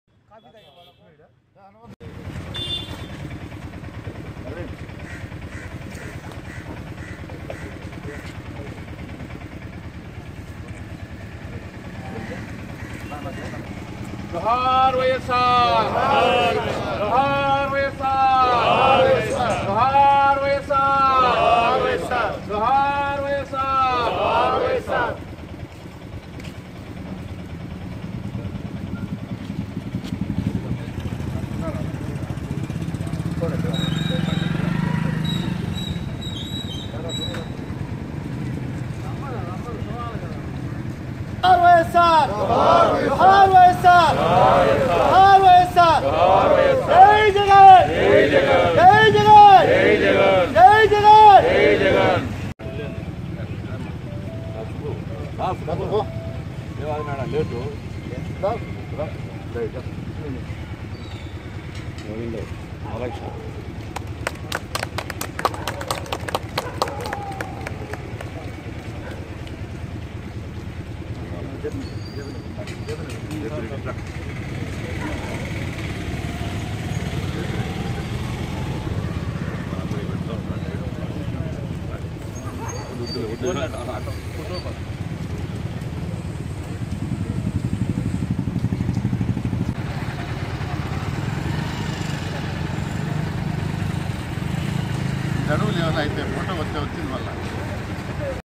YSRCP Leaders celebrate Dr.YSR 71st Birthday celebrations at Rajampet
YSRCP Leaders celebrate Dr.YSR 71st Birthday celebrations at Rajampet - 8th July 2020